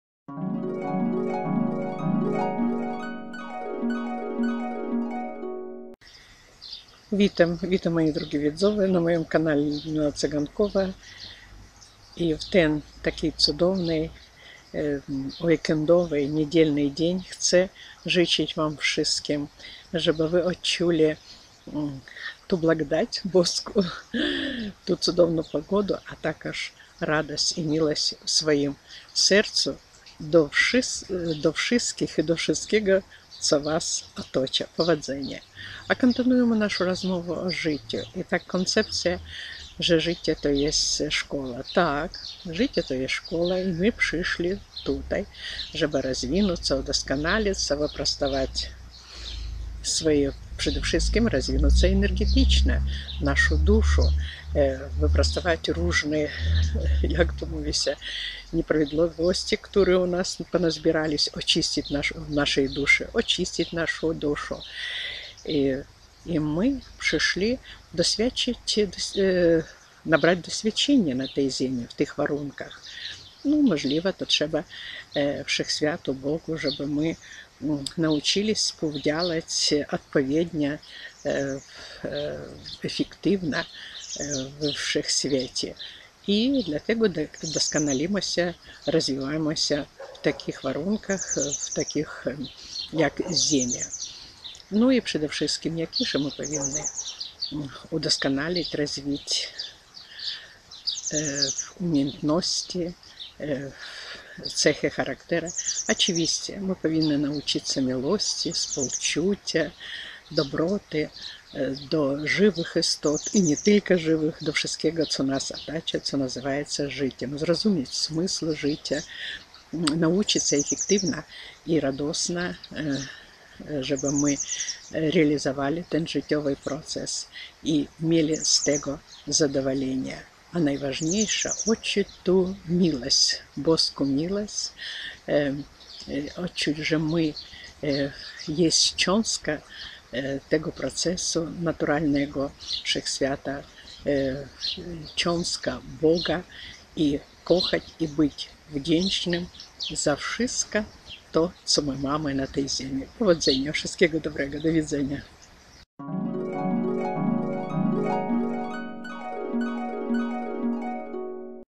ŻYCIE-CUDOWNA, MAGICZNA SZKOŁA
Na wideo-Życie-Cudowna, magiczna szkoła- autorka rozwija koncepcje swego życia.